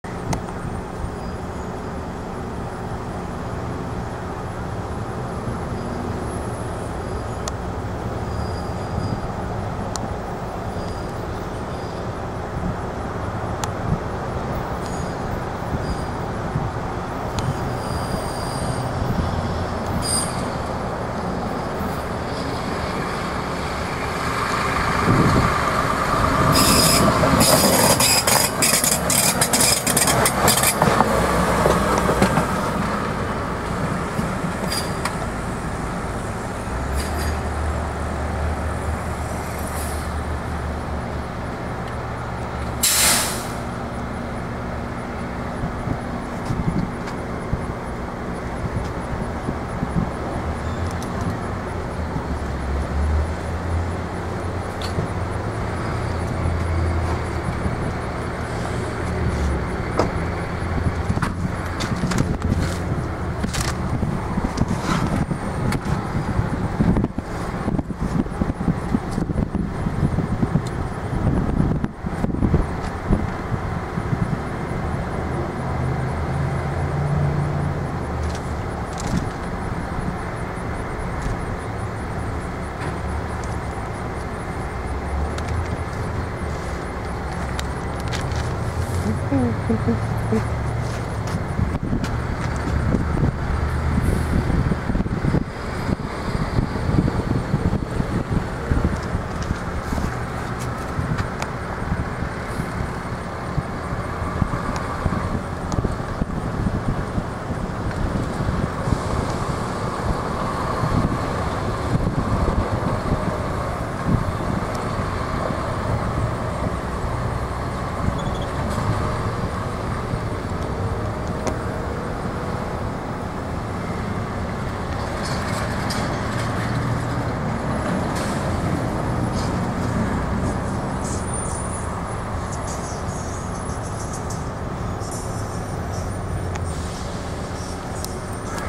Norfolk Southern High Railer Utility Truck (Work Truck) In(HD) Austell,Ga. 7-22-2013
Norfolk Southern Work Truck High Railer With Friendly Crew Waves At Me .
3:55pm Monday.